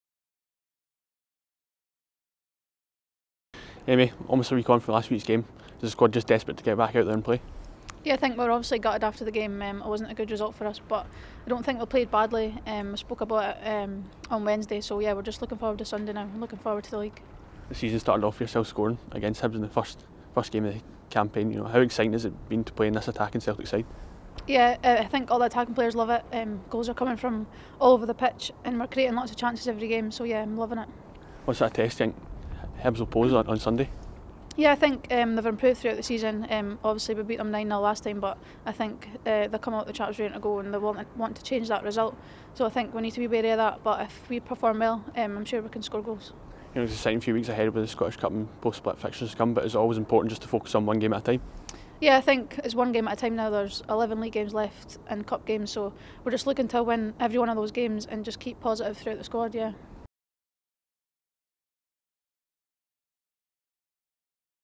Match preview with Amy Gallacher | Hibernian v Celtic FC Women
Amy Gallacher previews Sunday's game against Hibs